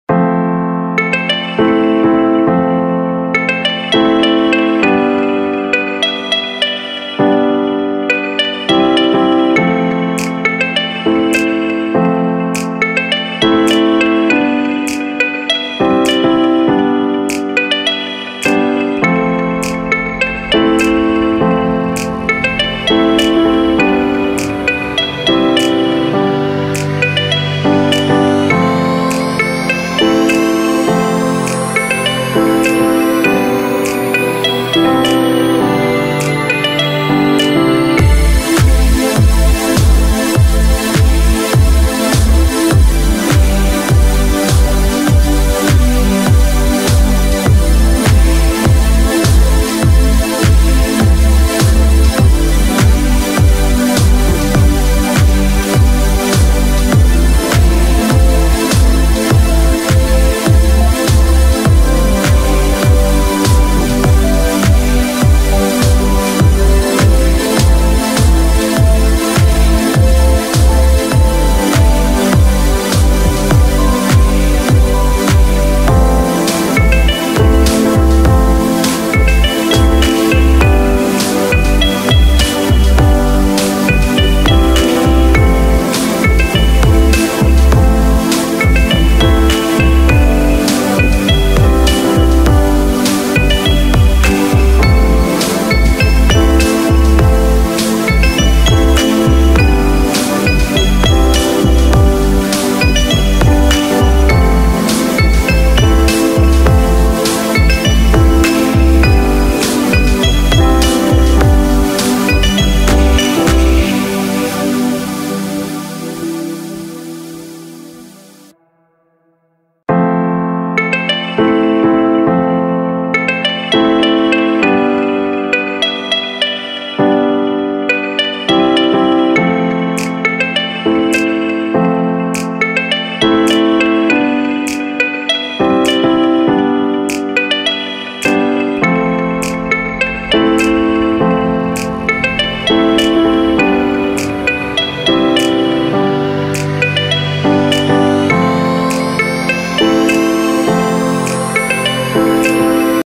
imágenes de paisajes bonitas con frases de la vida para reflexionar
En este vídeo te deleitaras viendo imágenes de paisajes bonitos, con frases de la vida para reflexionar, ademas con música de fondo para que lo disfrutes mas. Muchas gracias por su preferencia.